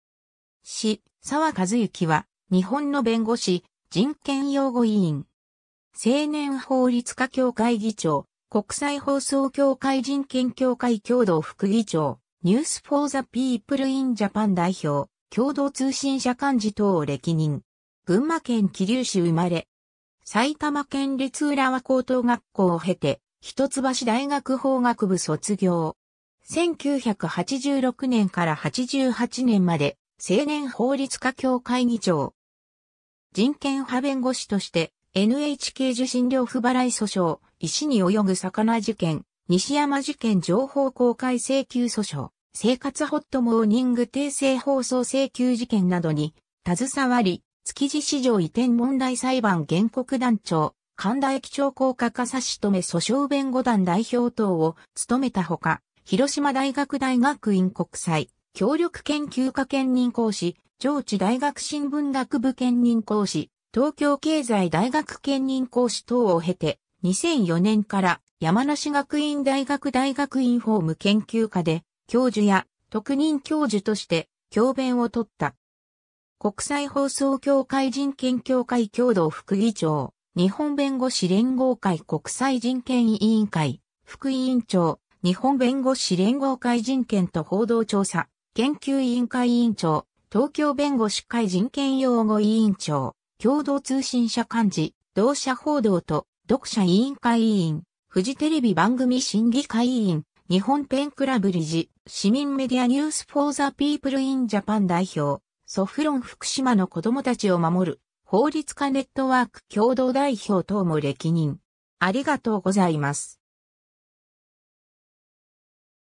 梓澤和幸
梓澤 和幸（あずさわ かずゆき、1943年 - ）は、日本の弁護士、人権擁護委員。
青年法律家協会議長、国際法曹協会人権協会共同副議長、News for the People in Japan代表、共同通信社監事等を歴任。
 群馬県桐生市生まれ。
埼玉県立浦和高等学校を経て、一橋大学法学部卒業。
1986年から88年まで青年法律家協会議長。
人権派弁護士としてNHK受信料不払い訴訟、石に泳ぐ魚事件、西山事件情報公開請求訴訟、生活ほっとモーニング訂正放送請求事件などに携わり、築地市場移転問題裁判原告団長、神田駅超高架化差止訴訟弁護団代表等を務めたほか、広島大学大学院国際協力研究科兼任講師、上智大学新聞学部兼任講師、東京経済大学兼任講師等を経て、2004年から山梨学院大学大学院法務研究科で教授や特任教授として教鞭を執った。
国際法曹協会人権協会共同副議長、日本弁護士連合会国際人権委員会副委員長、日本弁護士連合会人権と報道調査研究委員会委員長、東京弁護士会人権擁護委員長、共同通信社監事、同社報道と読者委員会委員、フジテレビ番組審議会委員、日本ペンクラブ理事、市民メディアNews for the People in Japan代表、SAFLAN 福島の子どもたちを守る法律家ネットワーク共同代表等も歴任。